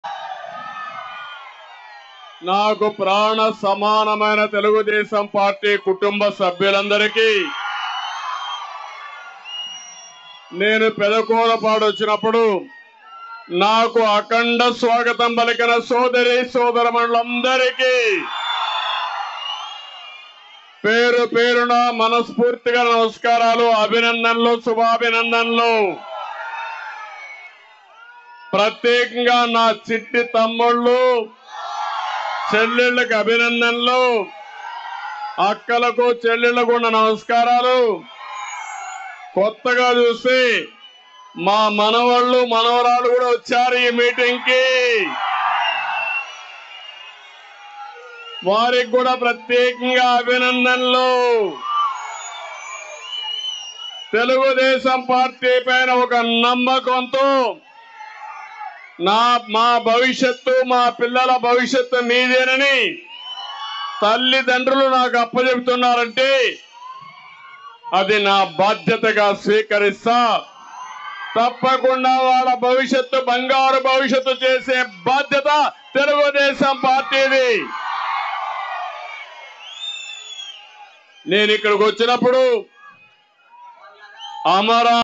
ఏపీ భవిష్యత్ నా బాధ్యత | Chandrababu Naidu About AP Future | TV5 News
LIVE : చంద్రబాబు ప్రసంగం | Chandrababu Public Meeting | TDP Pedakurapadu LIVE |  TV5 News